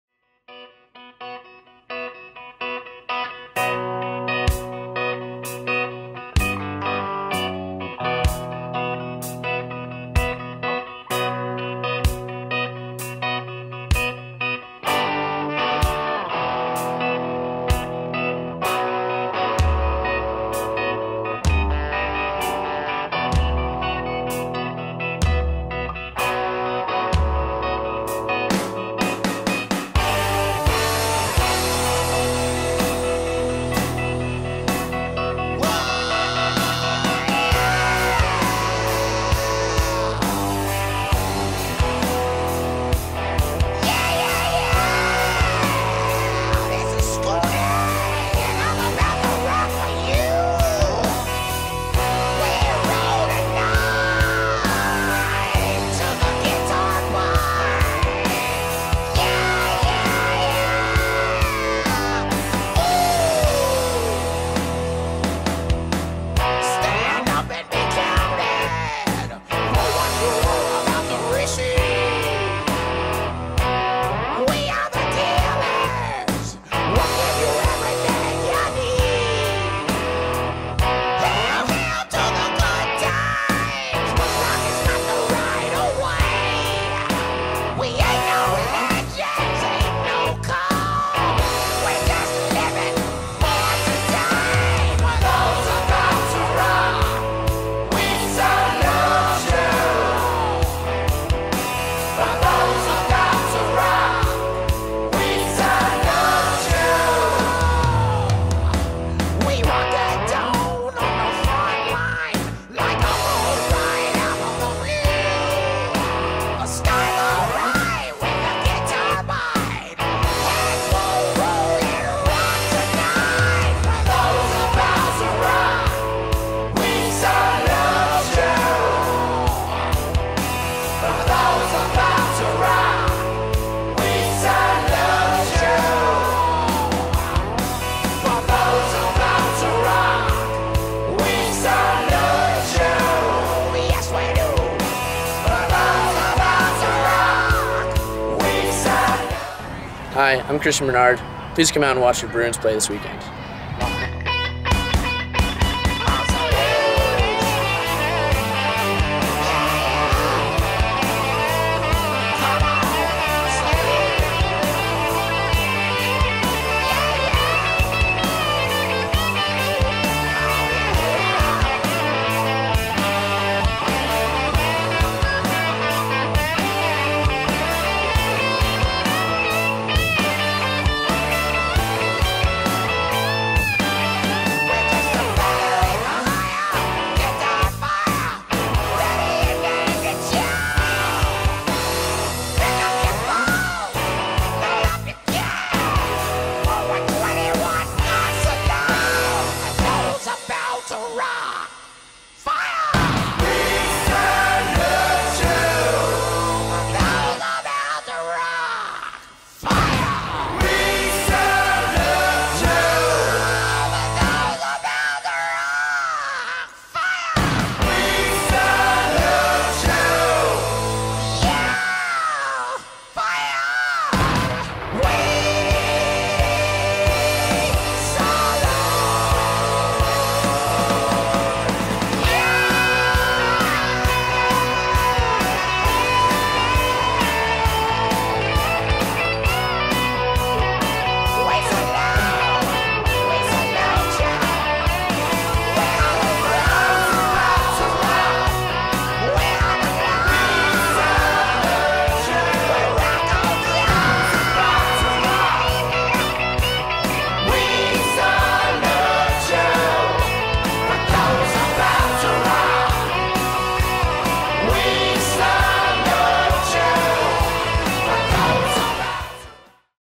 UCLA Rugby
Highlights from Saturday's warm-up games